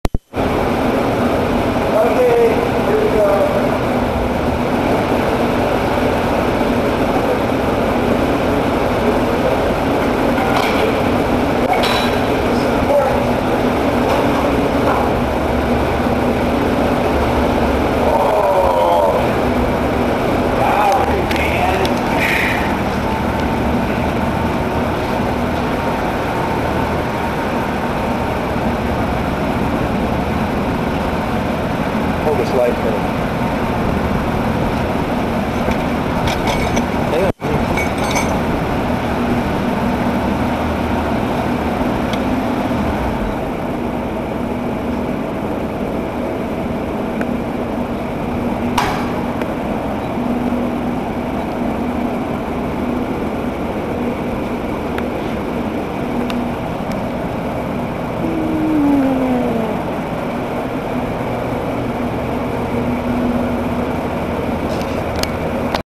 Porsche 911 clutch failure.
This is what happens when you don't use genuine porsche parts! Had a heavy duty racing pressure plate clutch disk. The failure was in the boss on the tranny where the TO bearing fork went through. Had to tear the tranny down and replace the case and the clutch The bill was about nine thousand dollars.